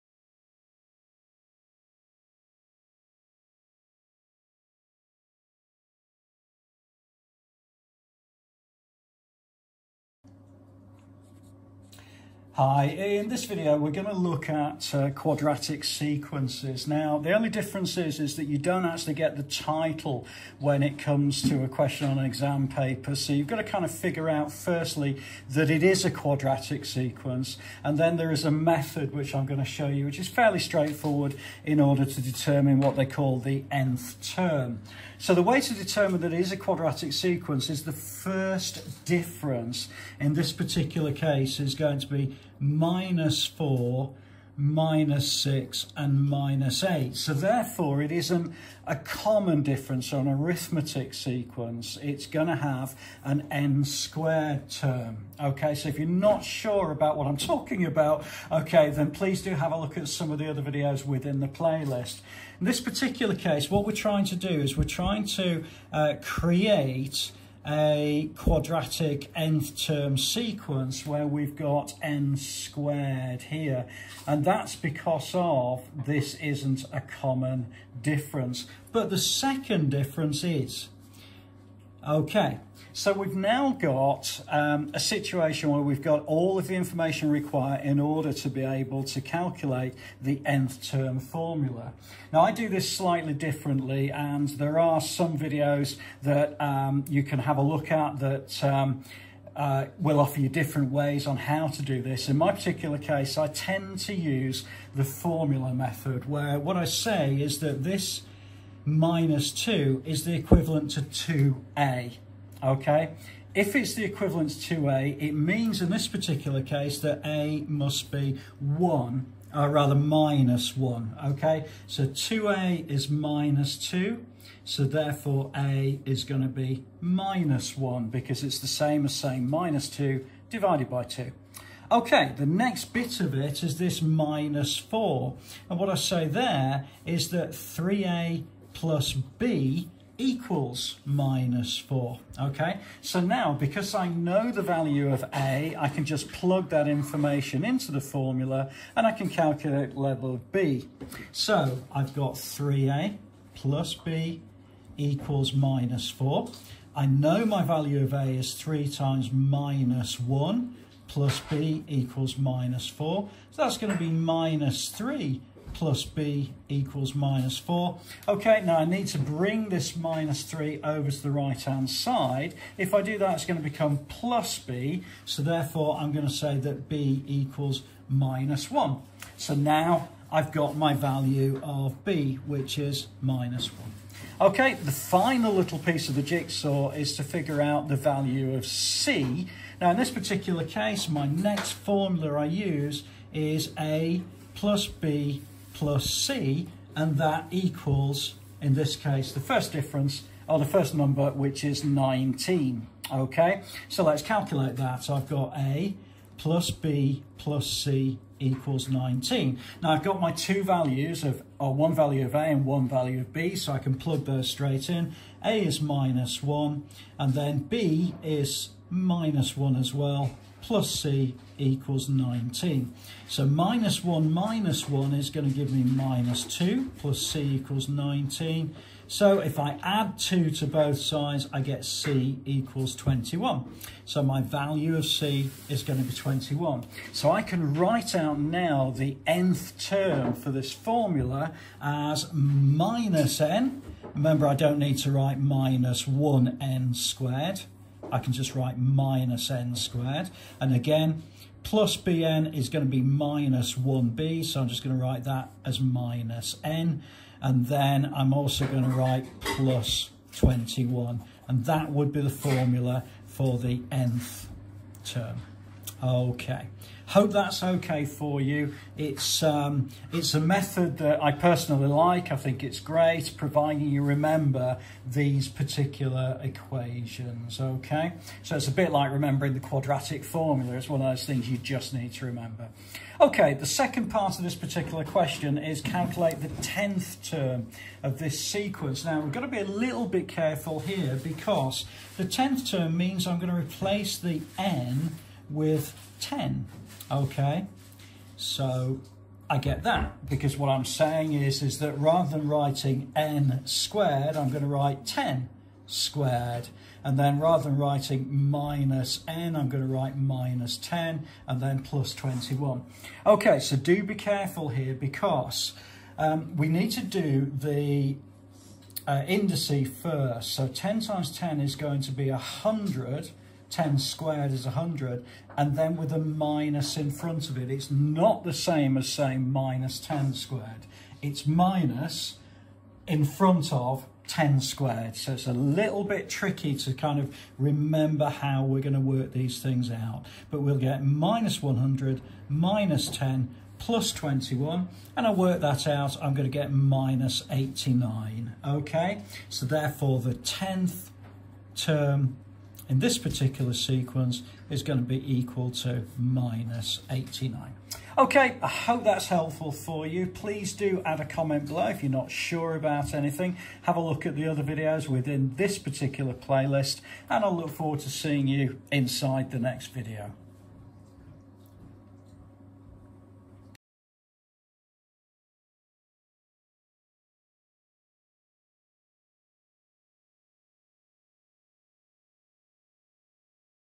How to find the nth term of a quadratic sequence 1 - the easy way
@3minutemaths for quick reminder and GCSE maths walkthrough videos.

This video is all about how to find the nth term of a quadratic sequence and forms part of the playlist of the same name.

The question is aimed at around grade 7+ GCSE maths:

a) Calculate the formula for the nth term of the following sequence:
19,  15,  9,  1 ….
b) Calculate the 10th term in the sequence.